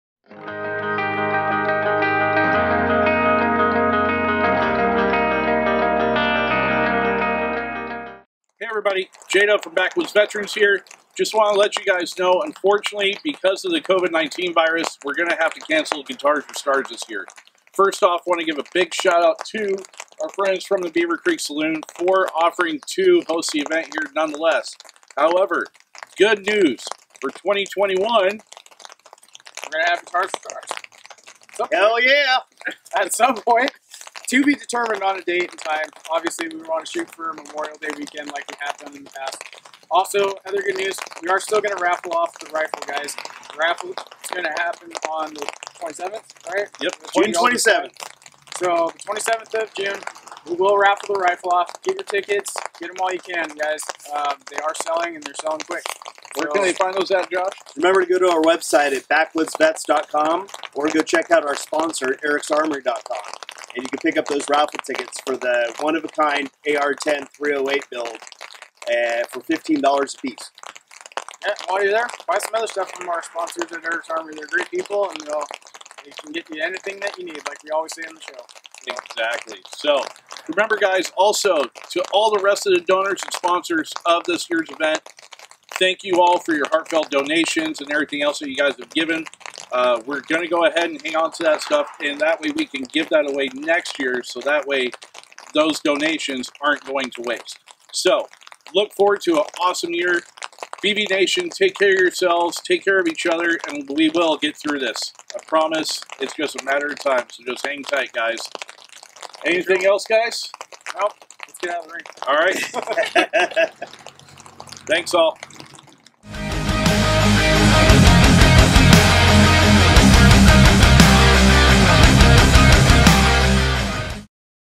Guitars For Stars will go on
it is with heavy hearts that we the directors of Backwoods veterans come to you today to announce that we will be canceling this years Guitars For Stars event due to state restrictions concerning COVID-19. we will do the event again in 2021, the raffle for the rifle will still be happening on June 27th so please head over to our website or Eric's Armory and get your raffle tickets.